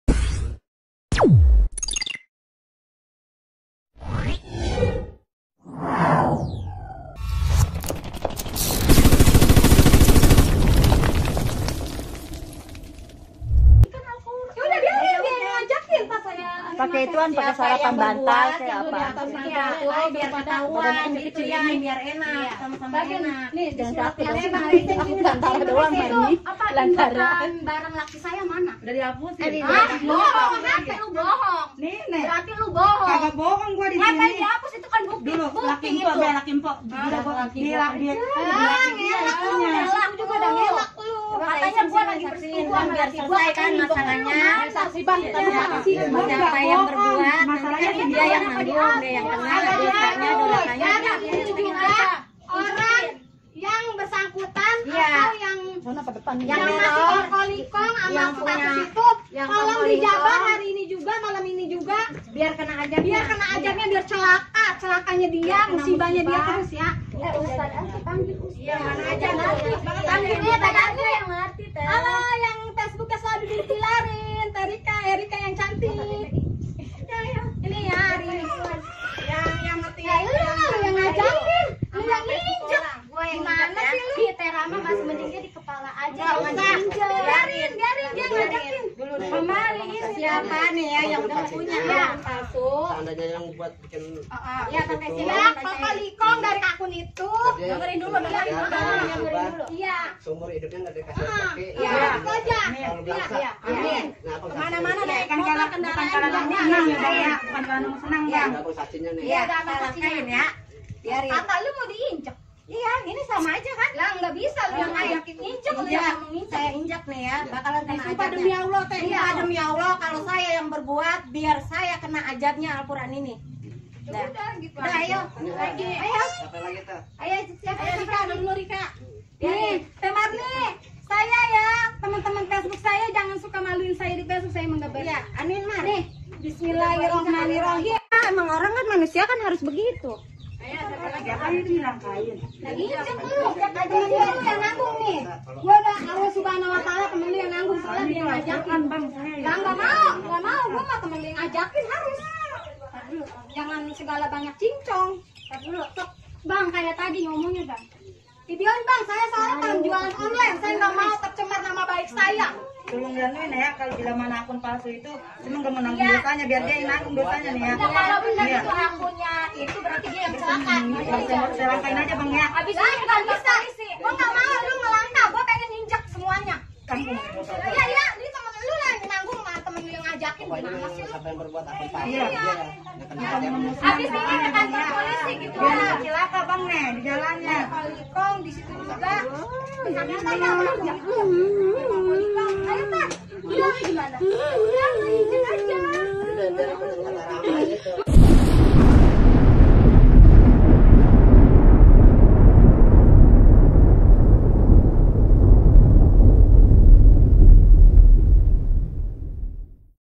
Geger | Beredar Video Beberapa Wanita Injak Al Qur'an | AmunisiTV
Geger | Beredar Video Beberapa Wanita Injak Al Qur'an
Diduga tindakan tersebut sebagai sumpah, para wanita tersebut rela menginjak Al Qur'an